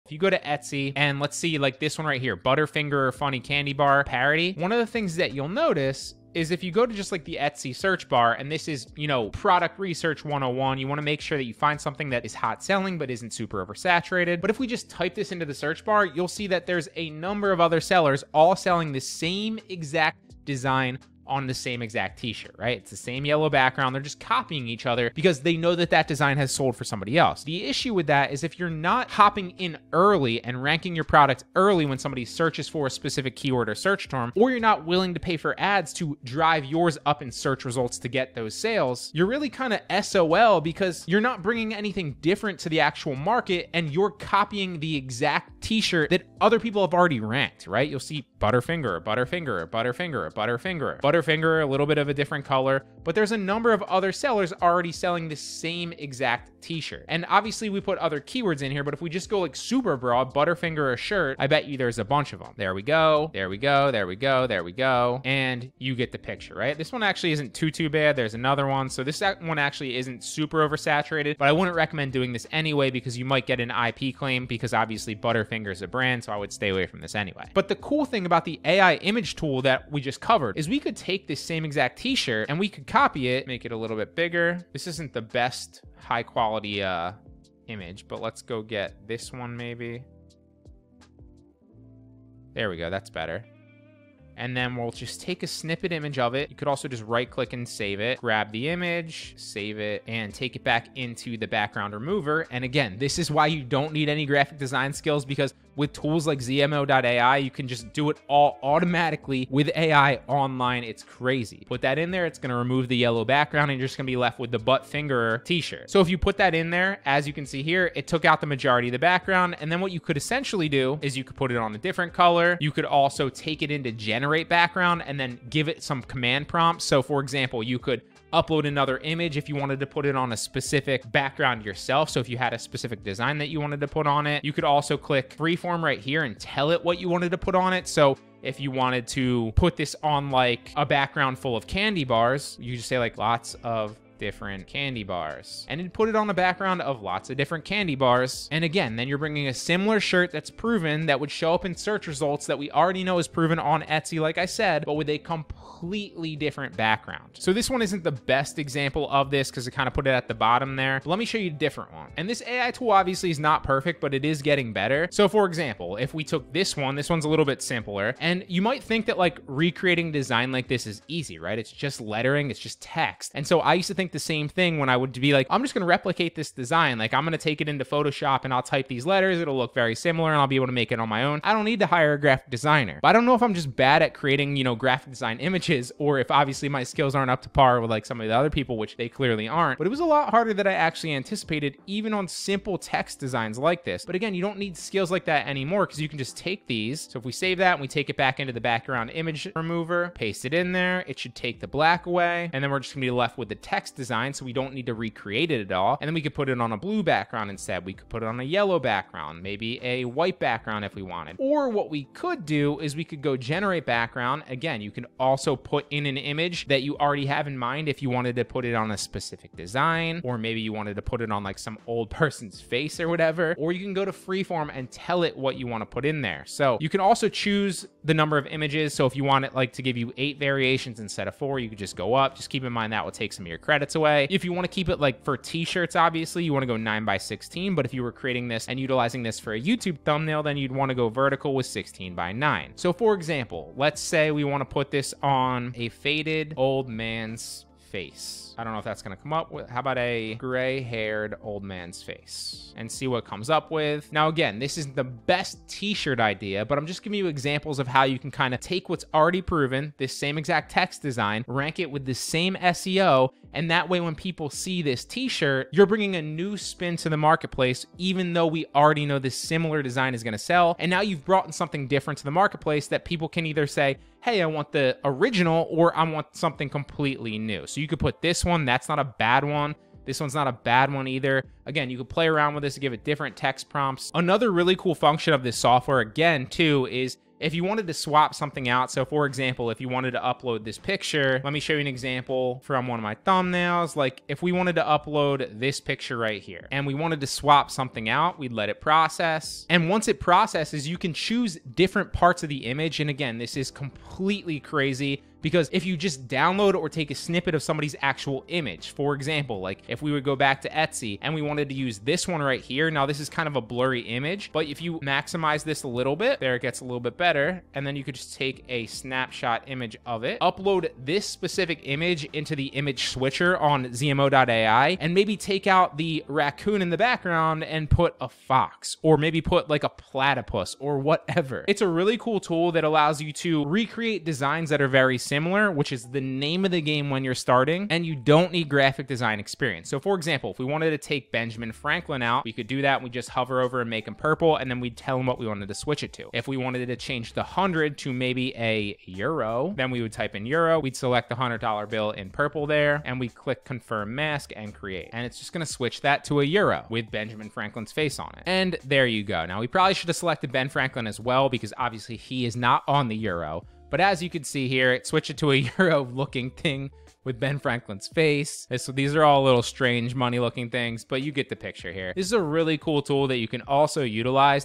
How I Create Print on Demand Designs Using ZMO AI Tool (Tutorial)
🔥Start Making Print on Demand Tshirts, Mugs, & More 100% FREE on Printful